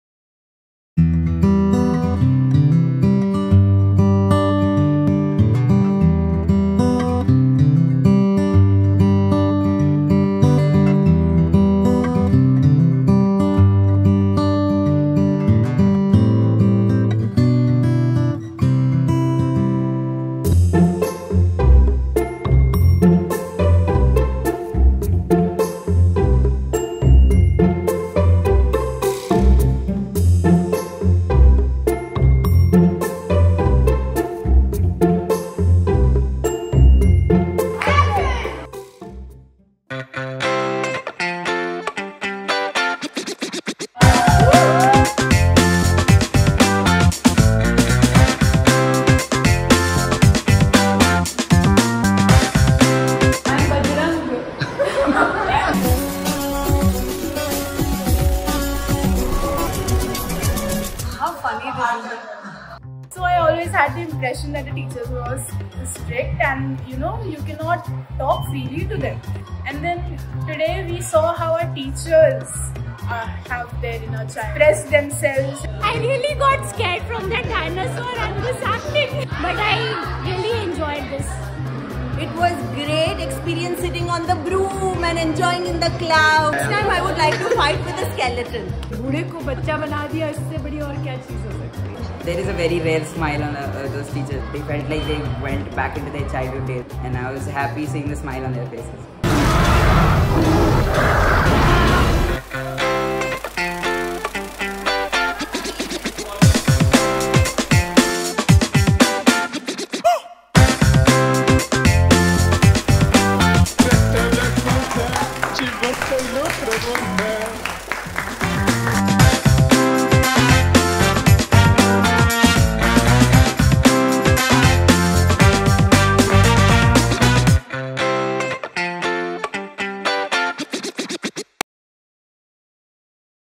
Happy Children’s Day || Fyule Video Lab with Sant Kabir International School
Children’s Day isn’t just about celebrating our children. It’s a day we remind ourselves to reconnect with the child within us.

So this time around, the Fyule Video Lab got together with the students Sant Kabir International School, to give its teachers a fun surprise. To help them get back in touch with their playful, natural, spontaneous self. To BecomeAChildAgain